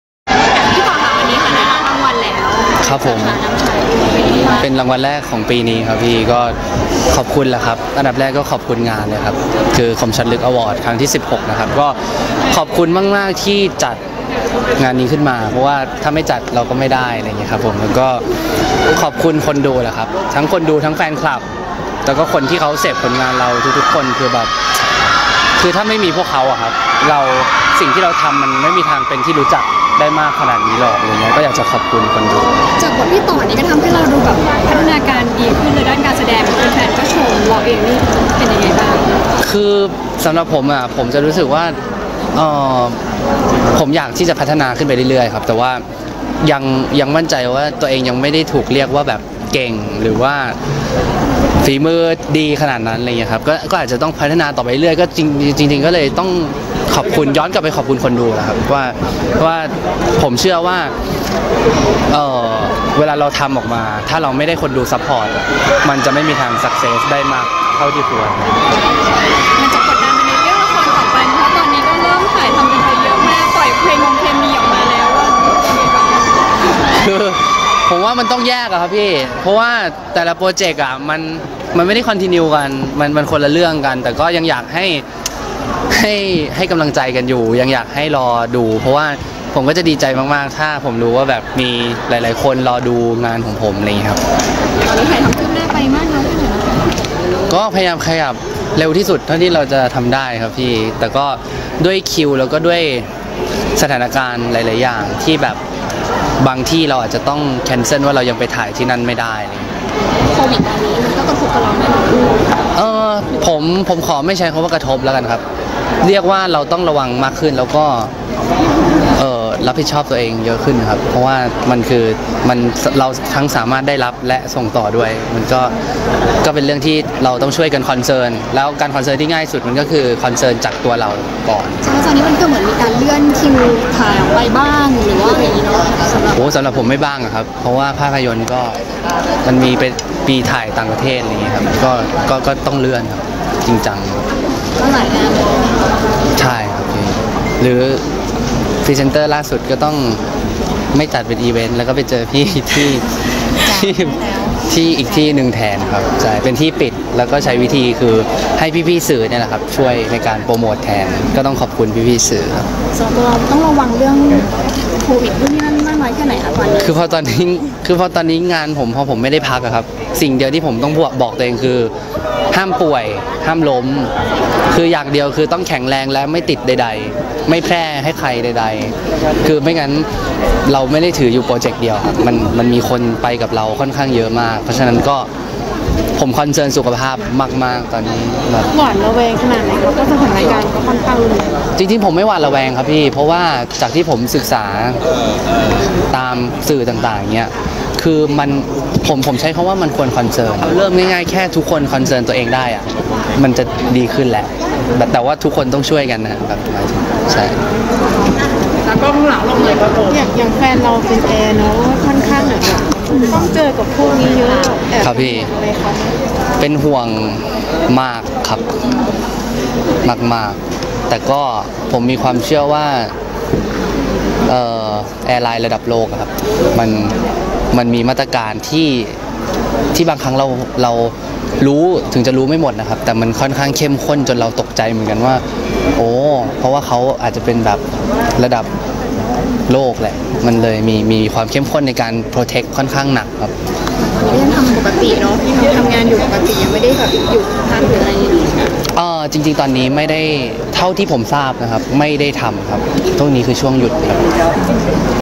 รางวัลแรกของปี! "ต่อ" ไม่เรียกตัวเองว่าเก่ง โอดพิษโรคระบาดเลื่อนหนังตปท. พรีเซ็นเตอร์งดจัดอีเวนต์
06/03/2020 รางวัลแรกของปี! "ต่อ" ไม่เรียกตัวเองว่าเก่ง โอดพิษโรคระบาดเลื่อนหนังตปท. พรีเซ็นเตอร์งดจัดอีเวนต์ ห่วง "มีน" แฟนแอร์ฯ
"ต่อ ธนภพ" ปลื้มคว้ารางวัลนำชายรางวัลแรกของปี ไม่เรียกว่าเก่ง แต่ได้รับการซัปพอร์ตที่ดี เผยโควิด-19 กระทบงาน เลื่อนหนังต่างประเทศ พรีเซ็นเตอร์งดจัดอีเวนต์ รับผิดชอบตัวเองมากขึ้น ห่วงแฟนแอร์โฮสเตส เชื่อบริษัทมีมาตรการเข้มข้น โชคดีช่วงนี้หยุดบิน